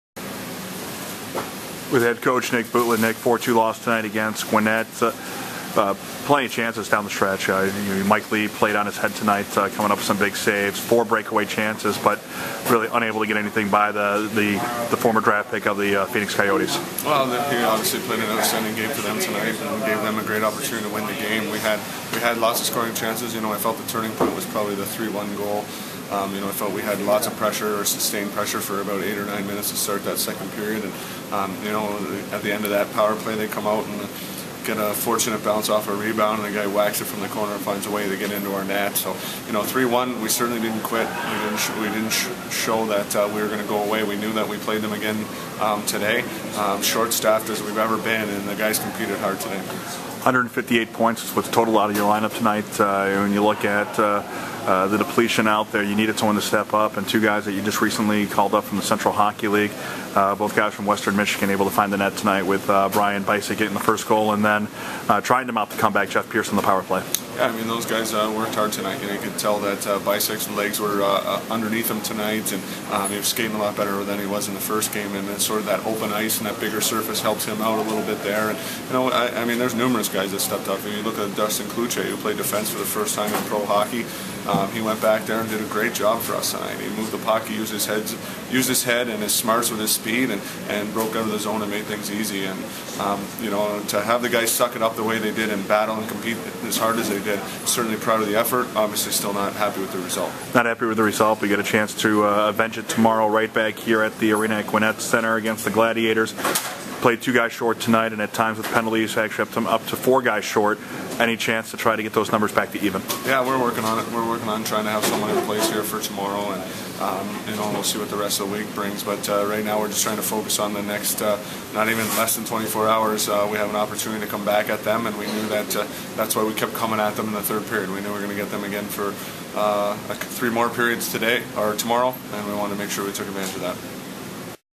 K Wings Game Recap 01 12 13
Head coach Nick Bootland talks about the K-Wings 4-2 loss to the Gwinnett Gladiators.